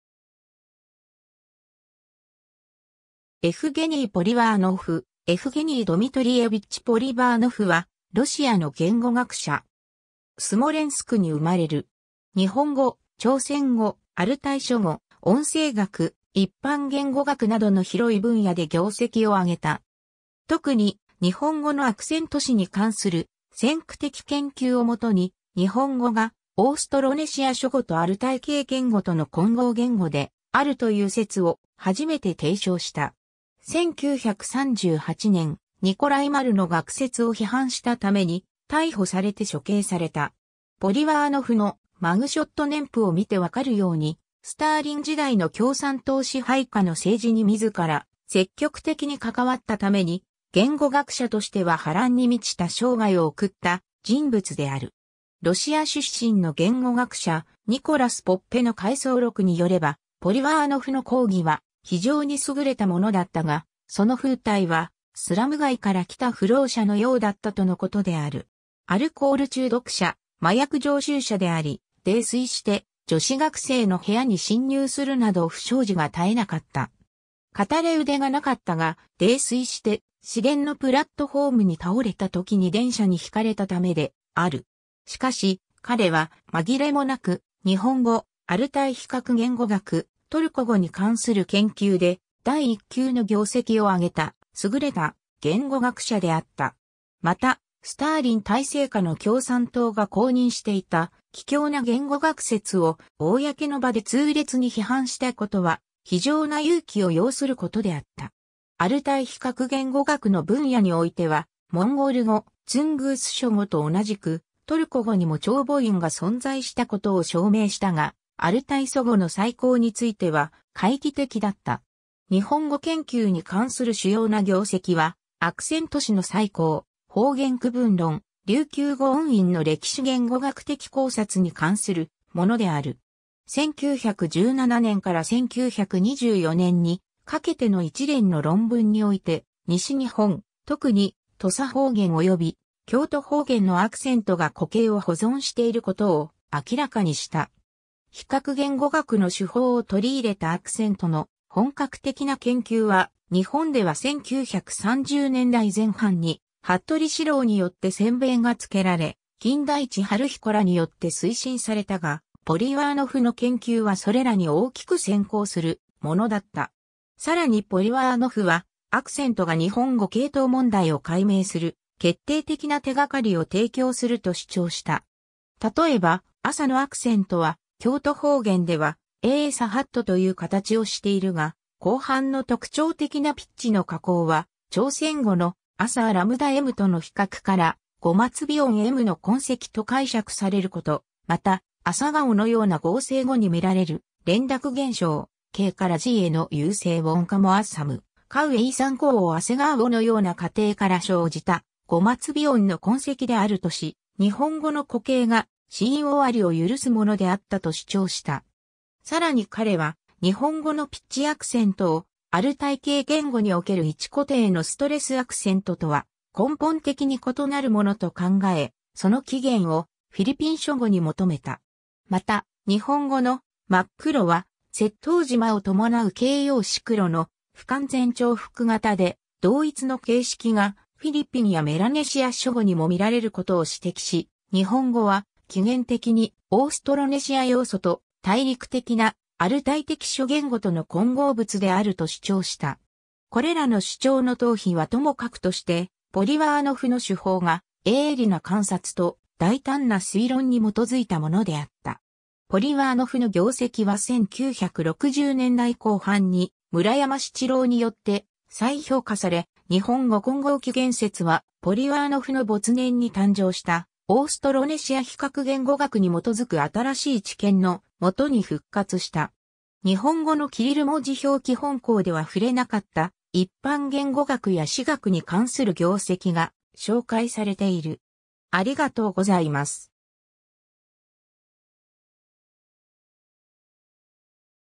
エフゲニー・ポリワーノフ
エフゲニー・ポリワーノフ エフゲニー・ドミトリエヴィッチ・ポリヴァーノフ（Евге́ний Дми́триевич Полива́нов、Yevgeny Dmitrievich Polivanov、1891年3月12日 - 1938年1月25日）は、ロシアの言語学者。
 スモレンスクに生まれる。
日本語、朝鮮語、アルタイ諸語、音声学、一般言語学などの広い分野で業績を挙げた。
特に日本語のアクセント史に関する先駆的研究を基に、日本語がオーストロネシア諸語とアルタイ系言語との混合言語であるという説を初めて提唱した。
1938年、ニコライ・マルの学説（スターリン体制における公式言語学説）を批判したために逮捕されて処刑された。
 ポリワーノフのマグショット 年譜を見て分かるように、スターリン時代の共産党支配下の政治に自ら積極的に関わったために、言語学者としては波乱に満ちた生涯を送った人物である。
 ロシア出身の言語学者、ニコラス・ポッペの回想録によれば、ポリワーノフの講義は非常に優れたものだったが、その風体は「スラム街から来た浮浪者」のようだったとのことである。
アルコール中毒者、麻薬常習者であり、泥酔して女子学生の部屋に侵入するなど不祥事が絶えなかった。
片腕がなかったが、泥酔して市電のプラットホームに倒れた時に電車に轢かれたためである。
 しかし、彼は紛れも無く、日本語・アルタイ比較言語学・トルコ語に関する研究で第一級の業績を挙げた優れた言語学者であった。
また、スターリン体制下の共産党が公認していた奇矯な言語学説を、公の場で痛烈に批判したことは、非常な勇気を要することであった。
アルタイ比較言語学の分野においては、モンゴル語、ツングース諸語と同じく、トルコ（テュルク）語にも長母音が存在した事を証明したが、アルタイ祖語の再構については懐疑的だった。
日本語研究に関する主要な業績は、アクセント史の再構、方言区分論、琉球語音韻の歴史言語学的考察に関するものである。
 1917年から1924年にかけての一連の論文において、西日本、特に土佐方言及び京都方言のアクセントが古形を保存していることを明らかにした。
比較言語学の手法を取り入れたアクセントの本格的な研究は、日本では1930年代前半に服部四郎によって先鞭が付けられ、金田一春彦らによって推進されたが、ポリワーノフの研究はそれらに大きく先行するものだった。
更にポリワーノフは、アクセントが日本語系統問題を解明する決定的な手掛かりを提供すると主張した。
 例えば、「朝」のアクセントは京都方言では a_(低)sa^(高低) という形をしているが、後半の特徴的なピッチの下降は、朝鮮語の「朝」 achΛm との比較から語末鼻音 m の痕跡と解釈される事、また「朝顔」(asagawo)のような合成語に見られる連濁現象( k からg への有声音化）も　asam+kawo 」 asaNkawo 」 asagawo のような過程から生じた語末鼻音の痕跡であるとし、日本語の古形が子音終わりを許すものであったと主張した。
 更に彼は、日本語のピッチアクセントを、アルタイ系言語における位置固定のストレスアクセントとは根本的に異なるものと考え、その起源をフィリピン諸語に求めた。
また、日本語の「真っ黒」（makkuro 「 ma+ku+kuro） は、接頭辞 ma を伴う形容詞 kuro の不完全重複形で、同一の形式がフィリピンやメラネシア諸語にも見られる事を指摘し、日本語は起源的に「オーストロネシア要素と大陸的なアルタイ的諸言語との混合物（アマルガム）」であると主張した。
 これらの主張の当否はともかくとして、ポリワーノフの手法が、鋭利な観察と大胆な推論に基づいたものであった。
ポリワーノフの業績は1960年代後半に村山七郎によって再評価され、日本語混合起源説は、ポリワーノフの没年に誕生したオーストロネシア比較言語学に基づく新しい知見の下に復活した。
 日本語のキリル文字表記 本稿では触れなかった一般言語学や詩学に関する業績が紹介されている。